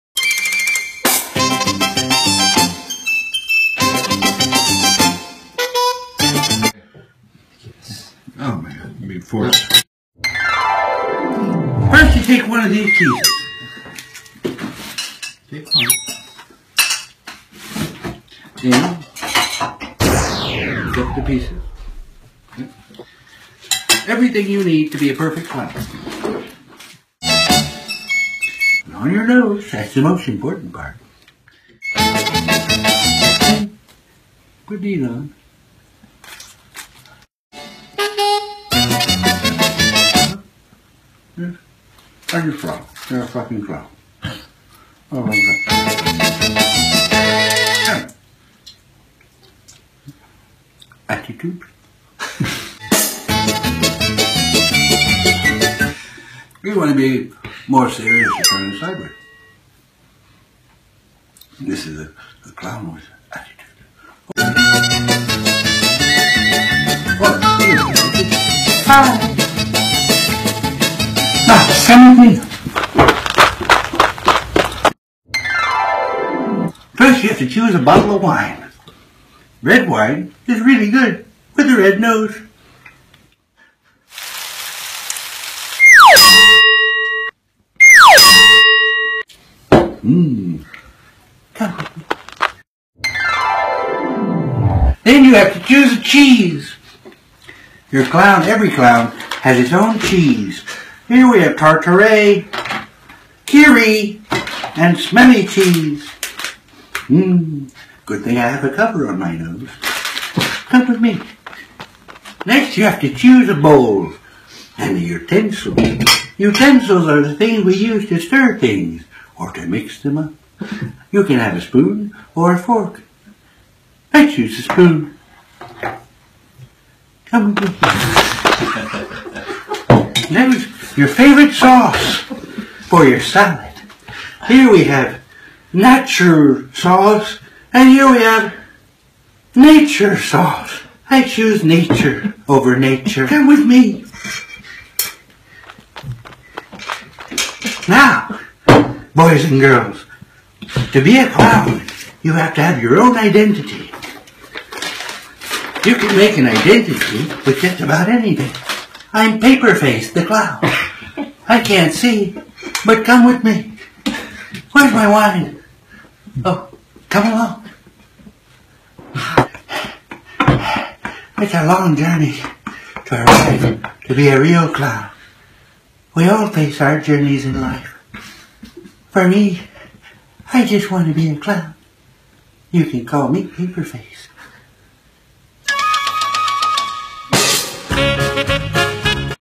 Jef Johnson - "How to Be a Clown" (The Best Tutorial Ever)
This parody of the all too cliche tutorial style "How to Be a Clown" videos was filmed impromptu in Lyon, France. Oct 2017.

photography/editing:  Frederic Boris Iuliani